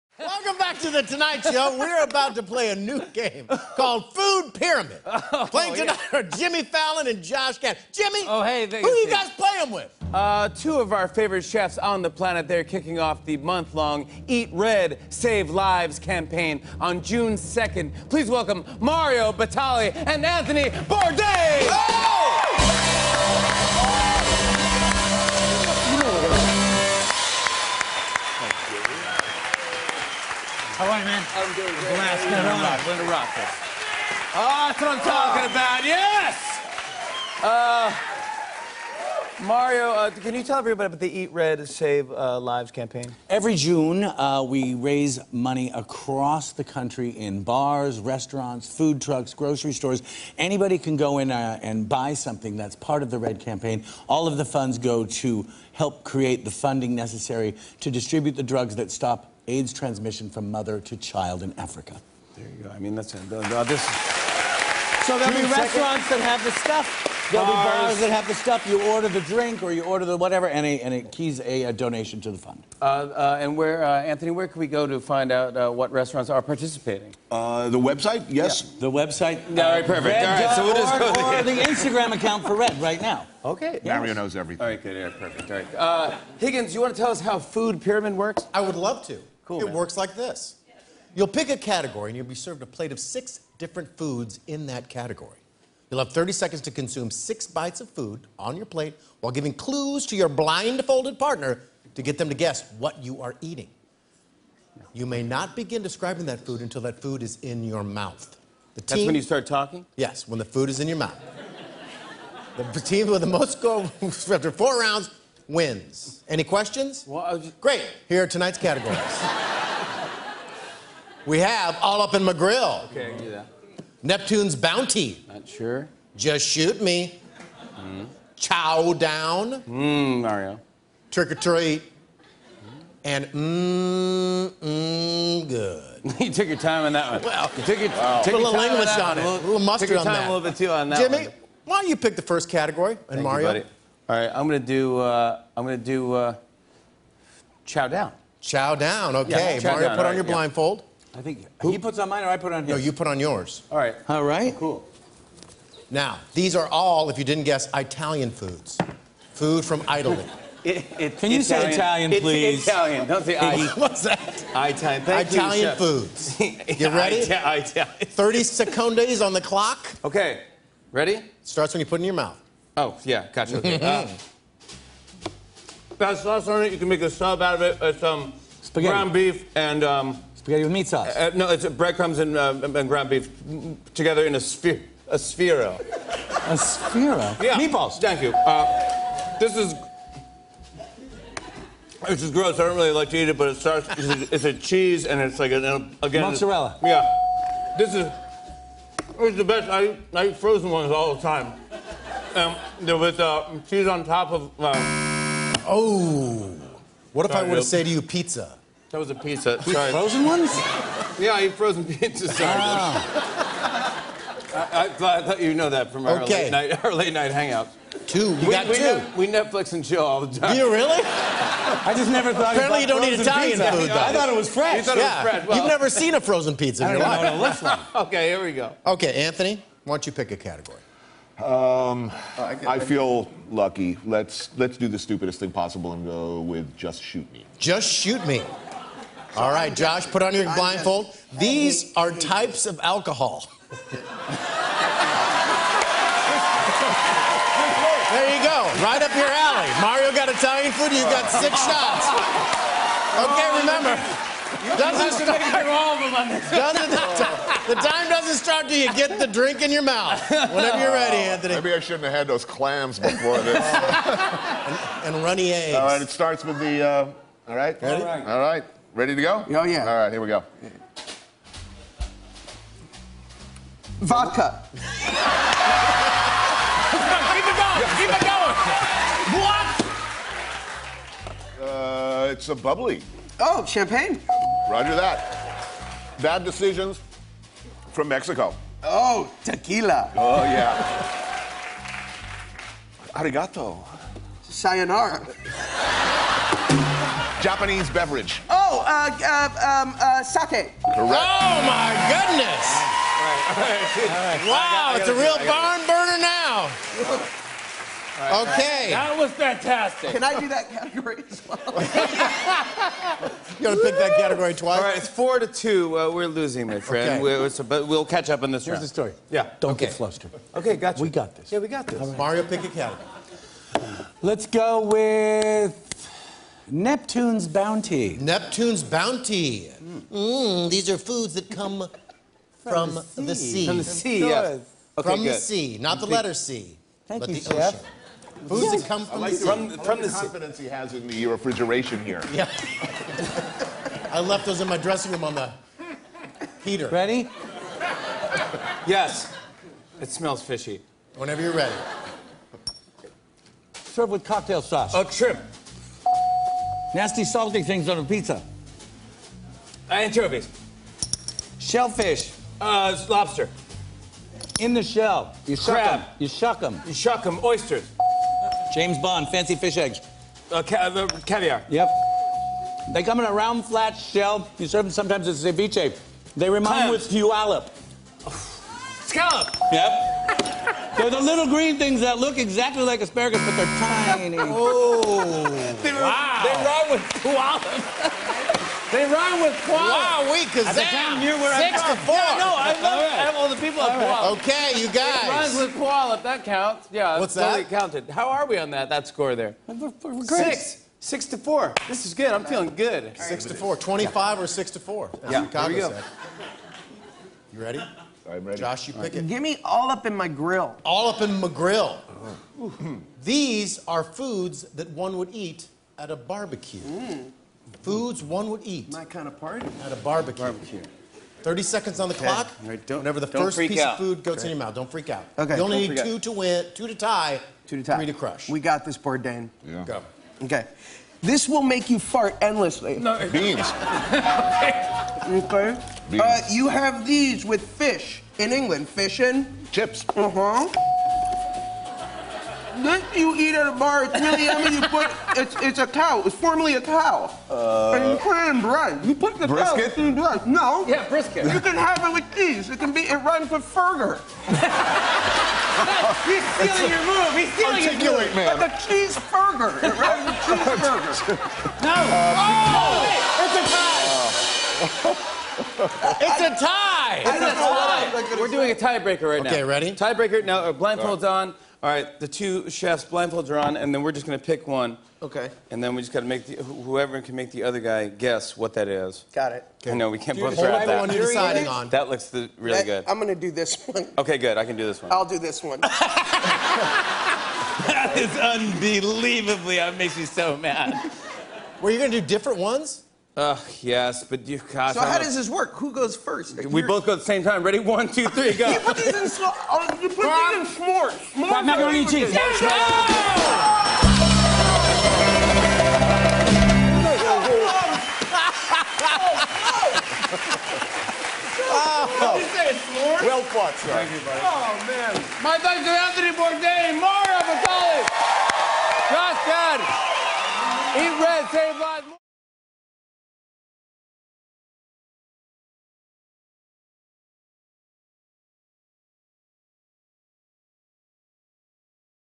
Food Pyramid with Anthony Bourdain, Mario Batali and Josh Gad
Jimmy and Mario Batali compete against Anthony Bourdain and Josh Gad in a food twist on the game Pyramid.